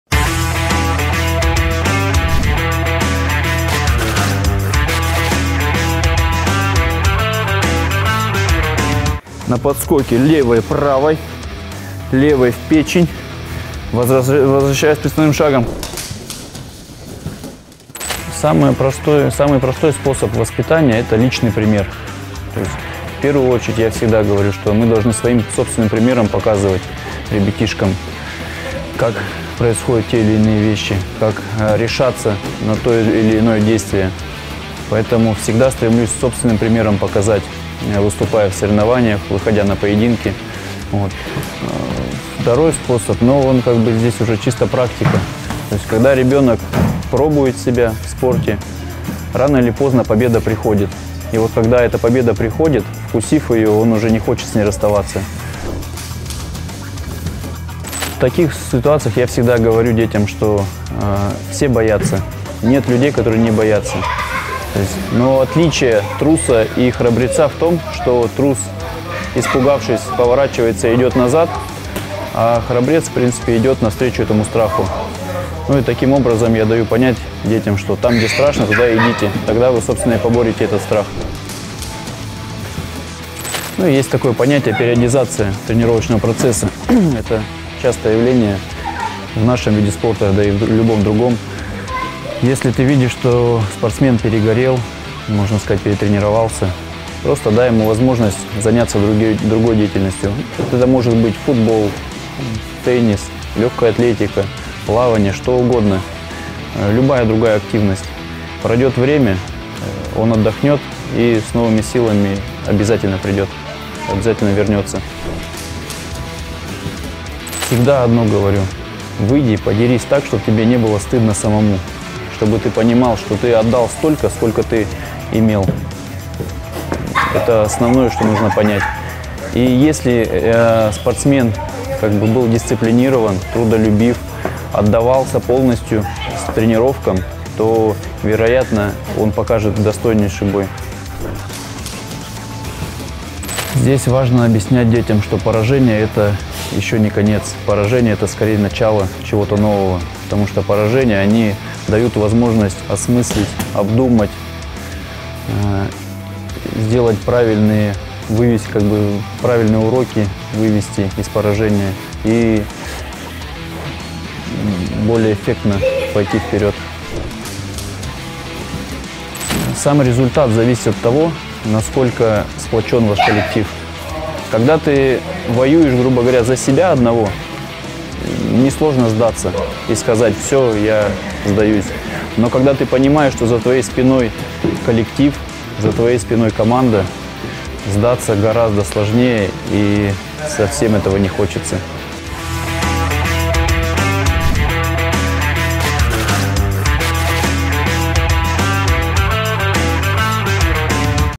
Центр смешанных единоборств "Топ Тим Сибирь" | ММА - философия спорта
Центр смешанных единоборств "Топ Тим Сибирь". ММА - что за вид спорта? Как дойти до олимпа ММА? Об этом и не только смотрите в наших видеоуроках